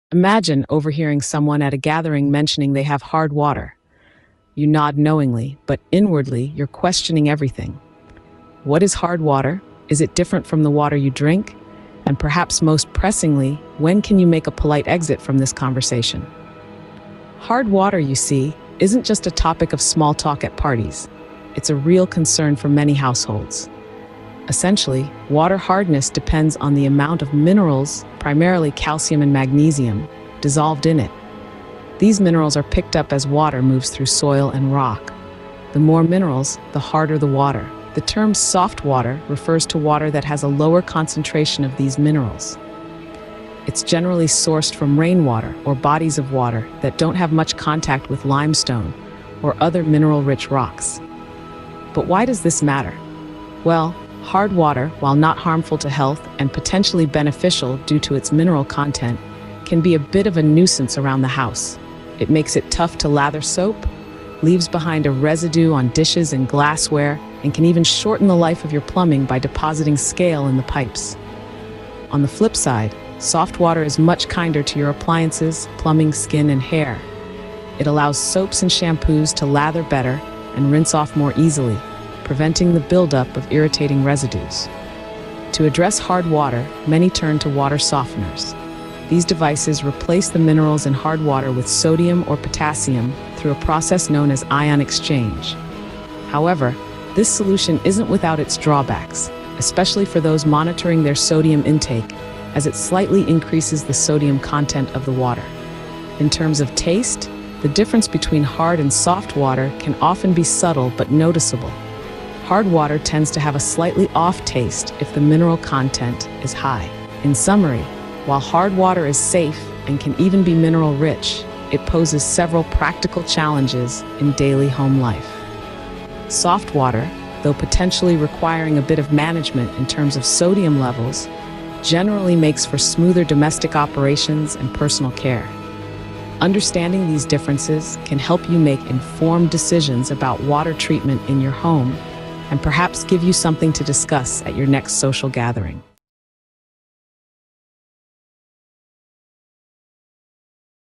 What’s the Difference Between Hard and Soft Water?
“Oh, I have hard water,” someone at a somewhat dull party might say, and you might nod, as though you understand the gravity of the situation. Then you may begin to wonder: Do I have hard water? What are the consequences of that? Is it drinkable? How soon can I leave this party?
So what exactly is “hard” water, and how does it actually differ from “soft” water? Also, what counts for just plain water?
Hardness is defined by the minerals the water contains as a result of how it’s sourced. Groundwater sourced near porous rocks, for example, will contain more minerals, while water from glaciers has almost none. If your water picks up calcium and magnesium in large amounts en route to your tap, it’s considered hard water. Health departments often assess hardness by measuring the milligrams of calcium carbonate per liter. According to the U.S. Geological Survey, anything below 60 mg/L is soft, up to 120 mg/L is moderately hard, 121 to 180 is is the hard stuff. Over 180 is considered very hard.
Is it harmful? No, but it can have annoying effects. Hard water is safe to drink and may even be beneficial to someone who is mineral-deficient. But that same water may prove too harsh for cleaning purposes, particularly when it comes to showering: The minerals can be irritating to skin and hair, stripping it of oils. It can also make washing soap off your hands harder, since the minerals reduce lather and increase soap scum.
It’s not just your hair and skin: Hard water can also lead to scale build-up in plumbing. And it can create the mineral spots you see after washing glassware. In general, it’s not ideal.
Soft water, on the other hand, is mostly free of these minerals, making it far less likely to be a problem for your skin, plumbing, or surfaces. That’s why some people choose to install water softeners that get rid of minerals by trapping them using resin beads and exchanging them for sodium or potassium.
It’s not a perfect solution: People watching sodium intake will have to account for the modest increase in sodium in their drinking water. But for the most part, folks tend to find soft water preferable to a water supply full of minerals. Because less water is needed for soap and detergents, it can also reduce your energy use.
(Don’t expect shower head filters to do much for minerals: These filters typically remove chlorine and general impurities but don’t have much effectiveness for hard water unless they specifically mention it.)
But what about the real question: Is there a taste difference? It’s a highly subjective question, but if the calcium is above 100 parts per million (ppm), you’re likely to notice something seems a little off. It’s another reason you may want to consider softening your supply. And if you want to see the hardness of the water supply in your area, check the USGS's map here.